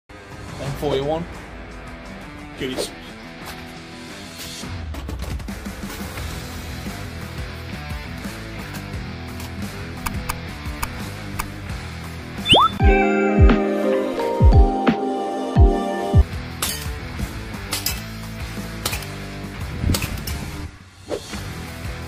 M4A1 BB Gun & Ghillie Suit Fun
Quick play and mess about with my m4a1 bb gun